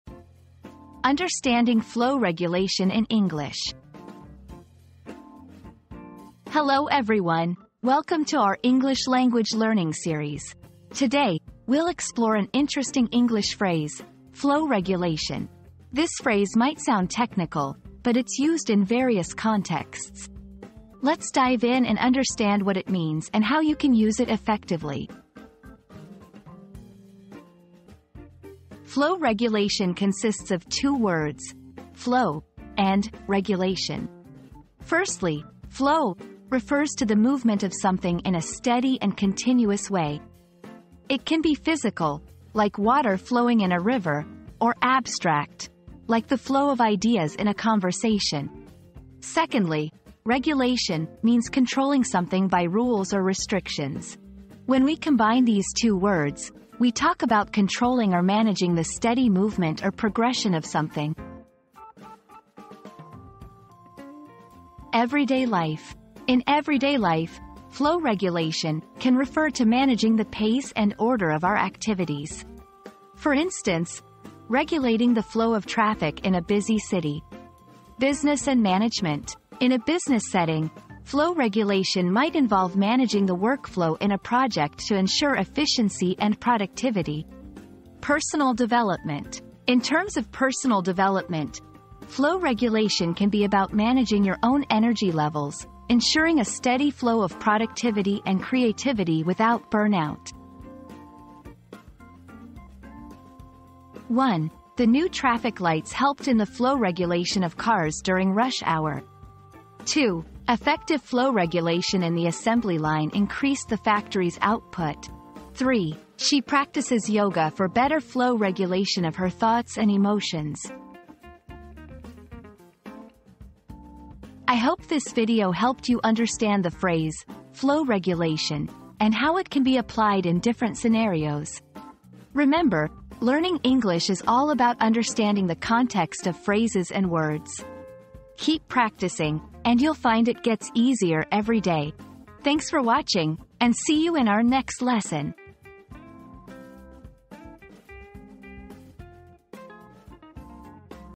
Understanding Flow Regulation in English
Unlocking the Secrets of English Flow Regulation • Discover the hidden techniques behind flow regulation in English and enhance your language fluency. From stress patterns to rhythm, learn how to master the art of smooth and natural English speech.

00:00 • Introduction - Understanding Flow Regulation in English
00:28 • Breaking Down the Phrase
01:04 • Contextual Usage of Flow Regulation
01:43 • Examples in Sentences